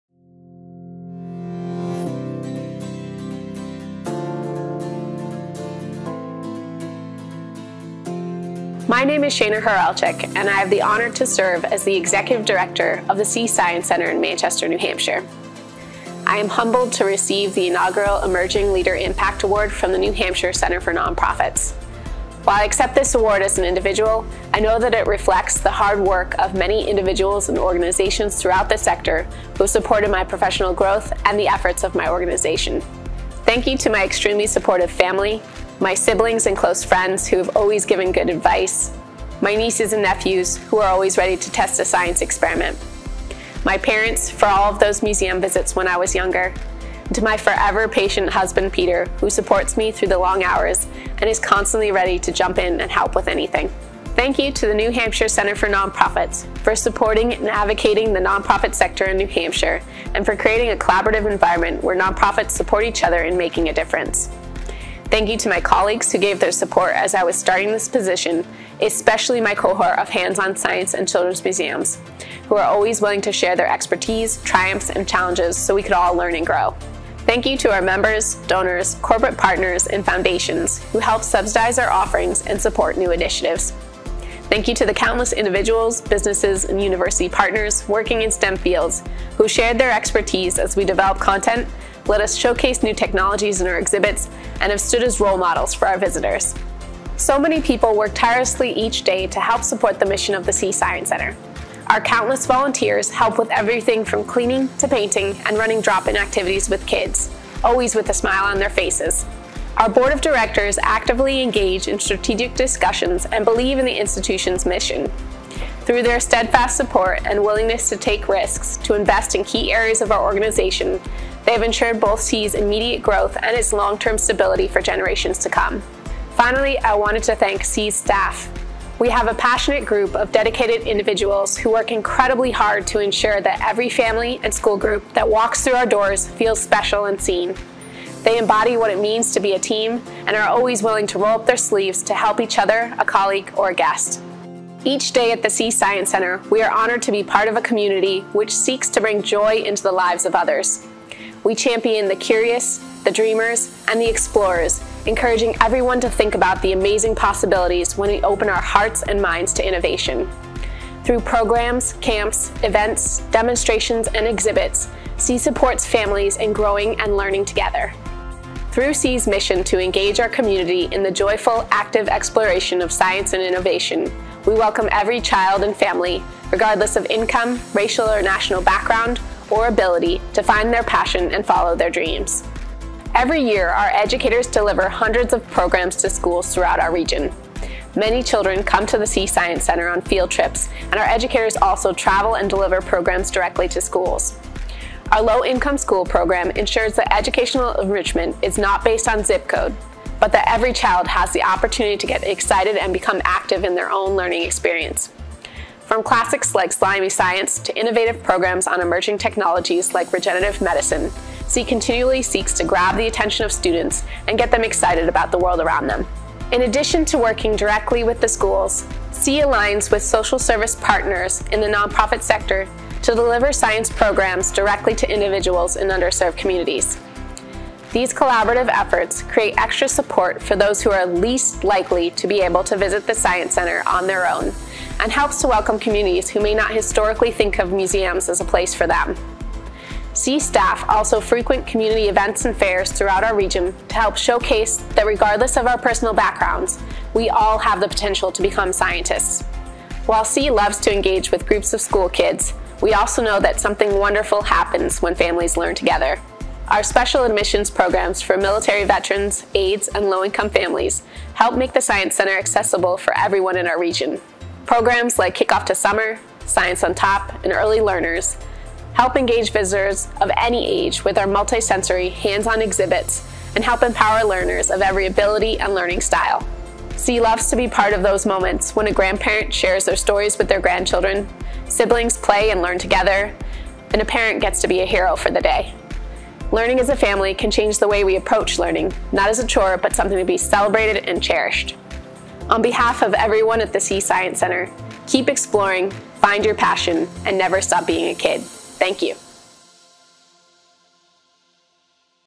SEE Executive Director Shana Hawrylchak is the 2024 Emerging Leader Impact Award winner
This video was presented at the NH Center for Nonprofits 2024 Nonprofit Impact Celebration in Concord, NH and celebrates the recent work done by the SEE Science Center.